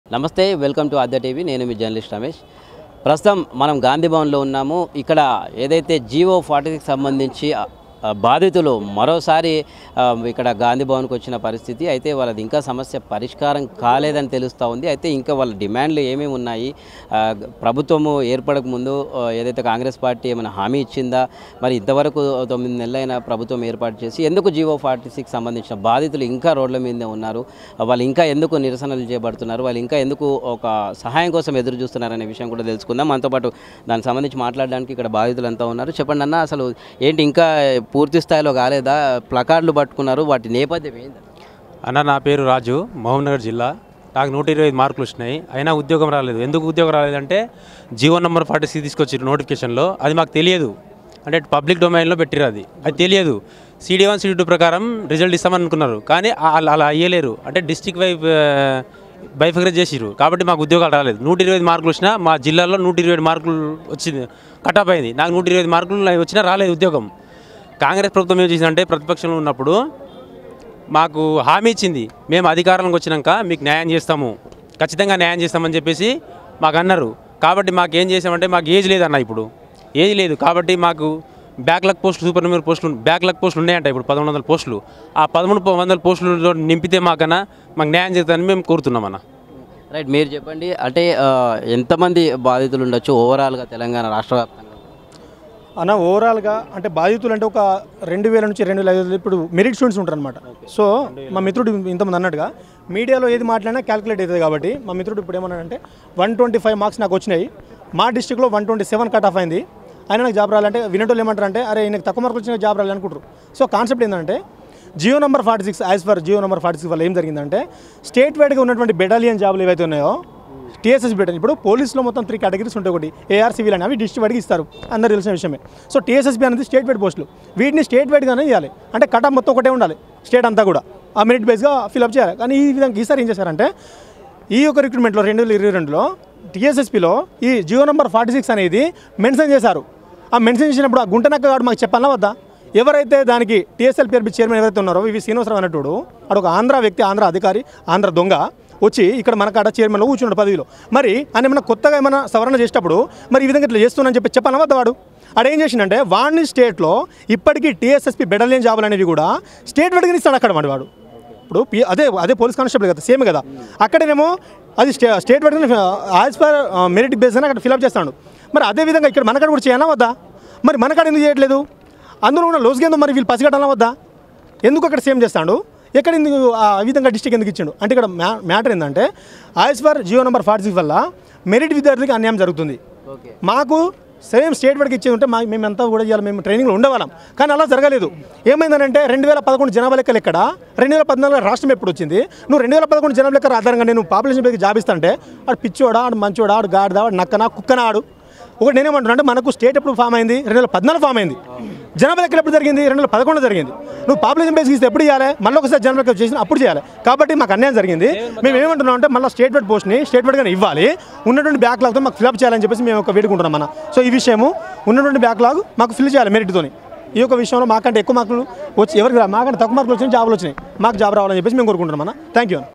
రెచ్చిపోయిన జీఓ46 బాధితులు | GO 42 Victims Protest at Gandhi Bhavan | Aadyatv
ఓరేయ్ ఆంధ్రా కుక్క.. అంతా నీ ఇష్టమేనా
టీఎస్ఎల్బీఆర్పీ జీఓ 46 తీసేస్తవా లేదా